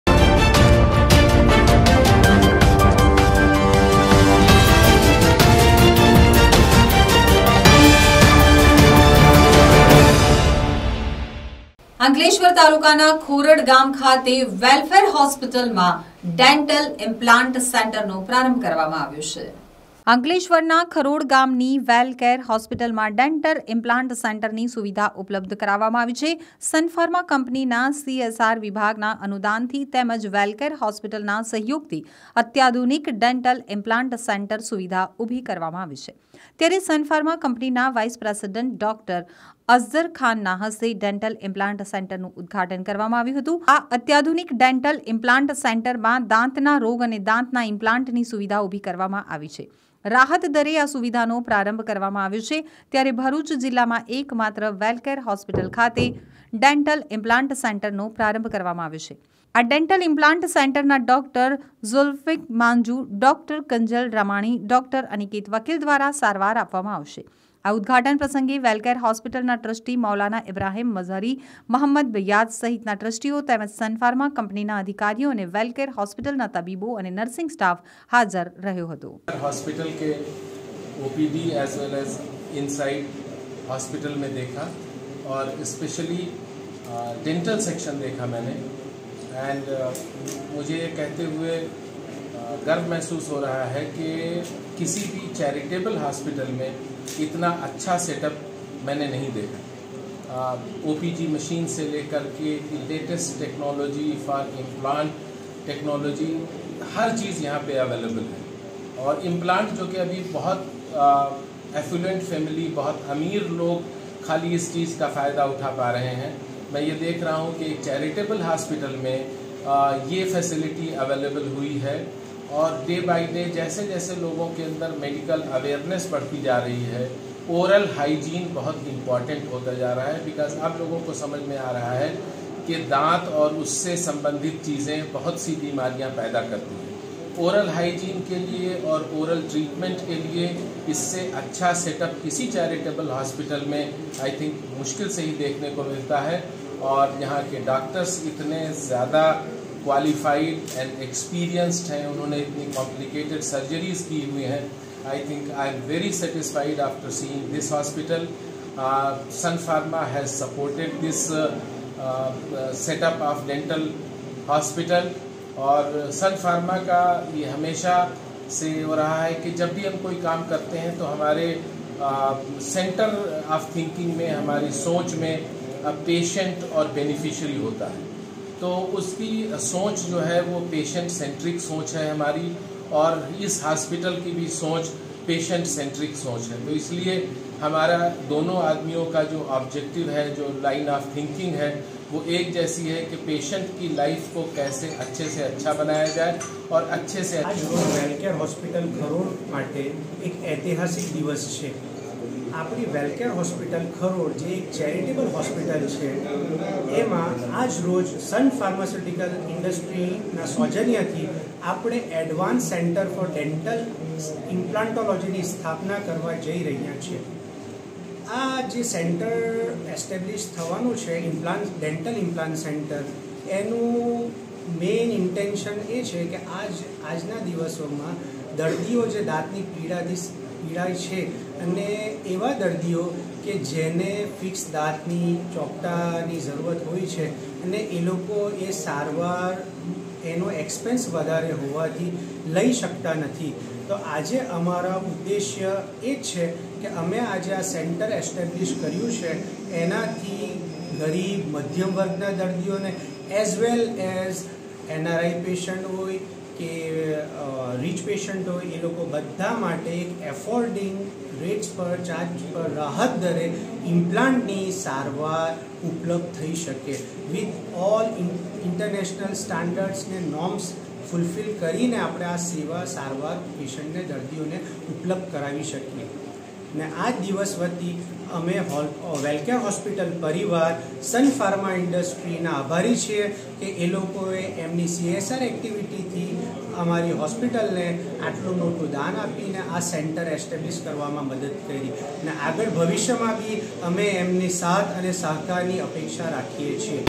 અંકલેશ્વર | અત્યાધુનિક ડેન્ટલ સેન્ટરનું સનફાર્મા કંપનીના વાઇસ પ્રેસિડેન્ટ ના હસ્તે ઉદ્ઘાટન
સનફાર્માના વાઇસ પ્રેસિડેન્ટ ડો.અઝદર ખાનના હસ્તે ઉદ્ઘાટન કરાયું